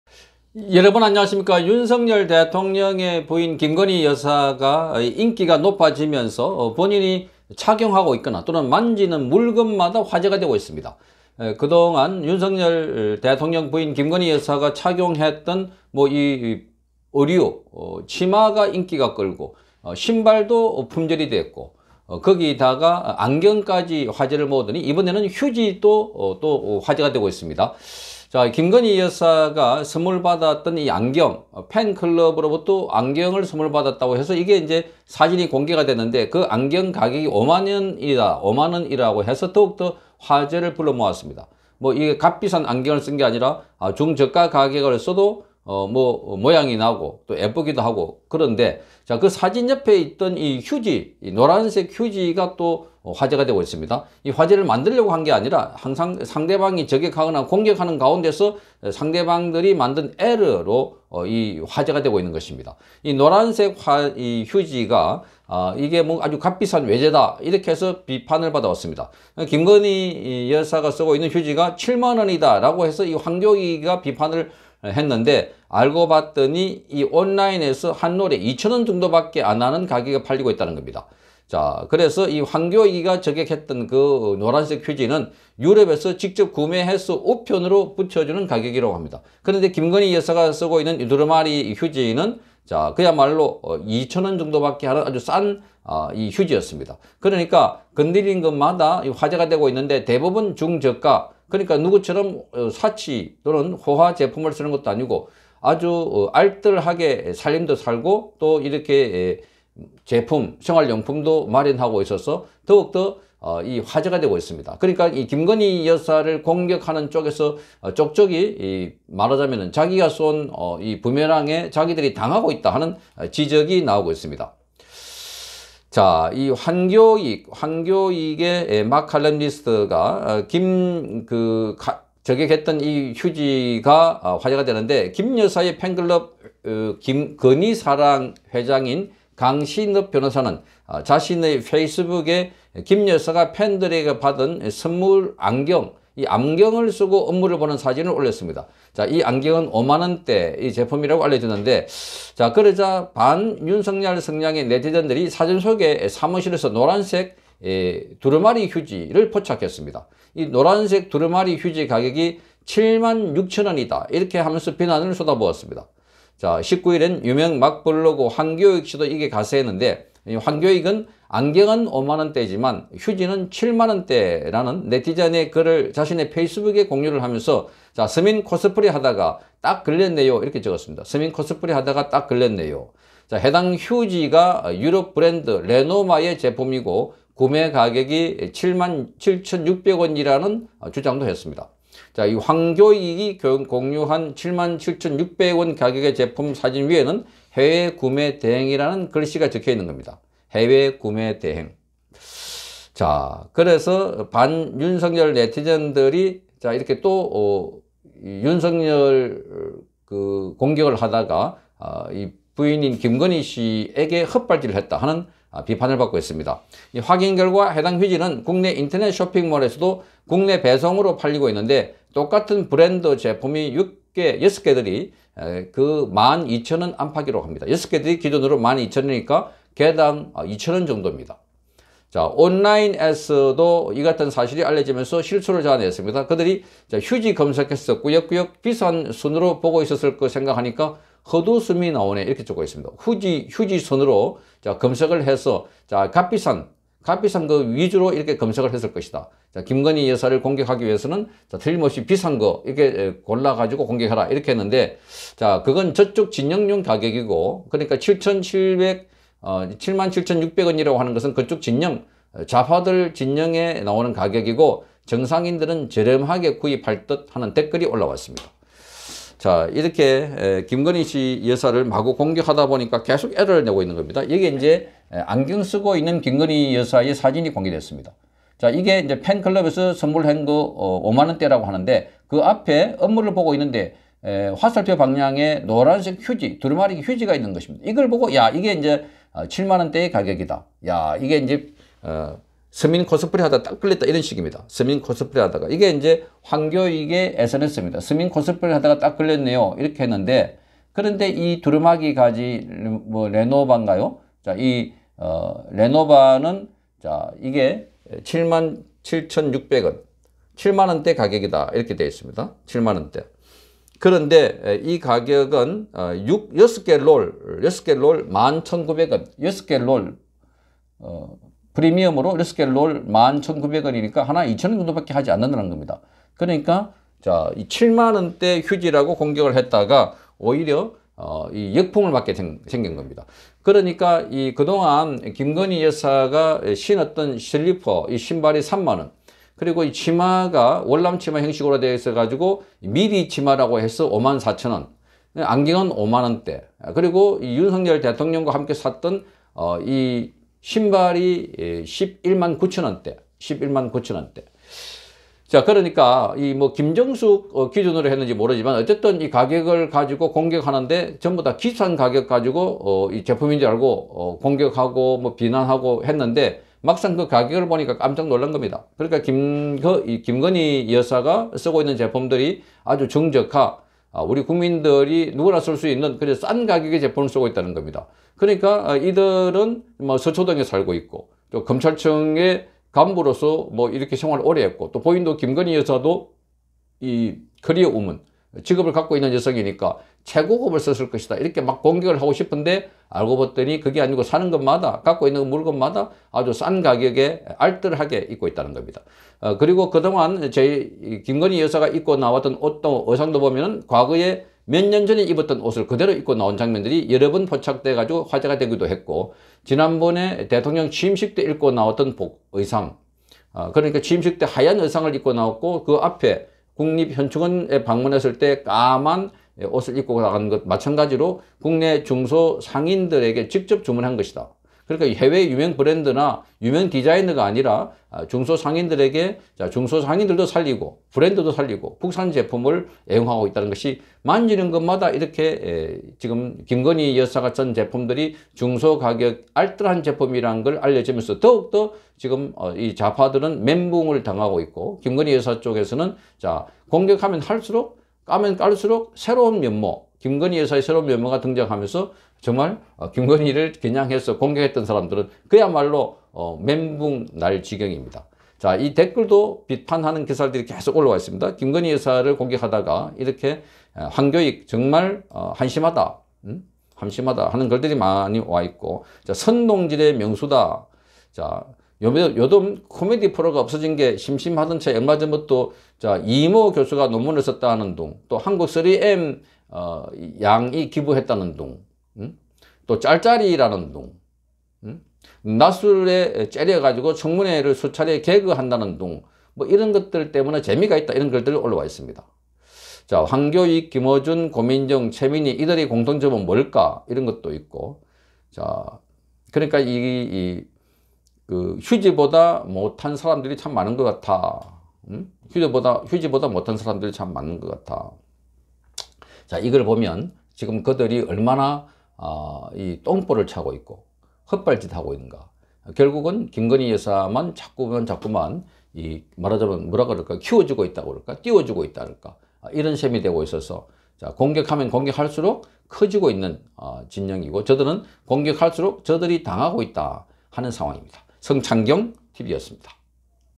“김건희 휴지가 7만원” 황교익 지적 ... 실제 가격 알고보니
김건희 사무실에서 사용하는 노랑휴지, 값비싼 제품이라며 "서민 코스프레" 주장하다가 황교익 폭망